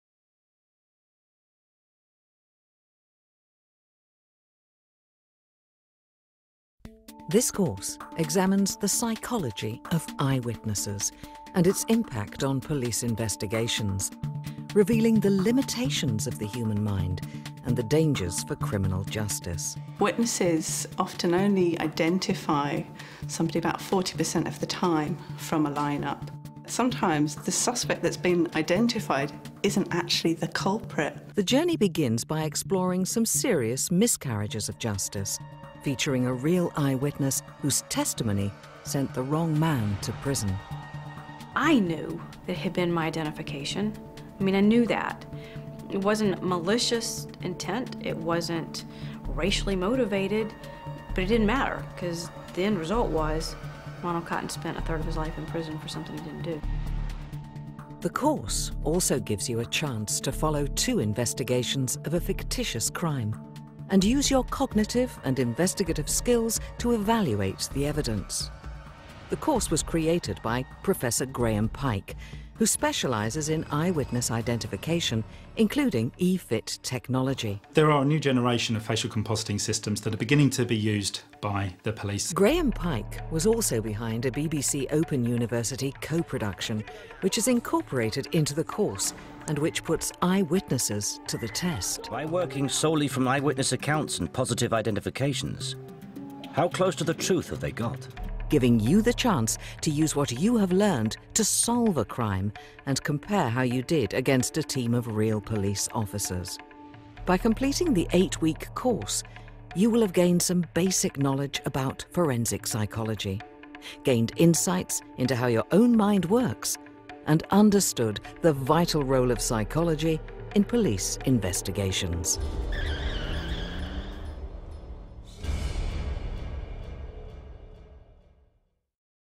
Forensic Psychology: Witness Investigation - free online course at FutureLearn.com
'Forensic Psychology: Witness Investigation' is a free online course by The Open University available on FutureLearn.com

Explore how your own mind works, and discover how the limitations of the human brain can lead to major miscarriages of justice.

Despite advances in forensic science, eyewitness testimony remains a critical component of criminal investigations. Psychological research has revealed the dangers of relying on evidence gained from an eyewitness and also how careful the police need to be when questioning witnesses.

Using videos of real witnesses and from cameras that go behind the scenes of a police investigation, this course explores the psychology of eyewitness testimony. You will get the chance to test your own cognitive skills and to see whether your powers of investigation are as good as a crack squad of police officers, as you try to solve a crime using nothing but evidence from eyewitnesses.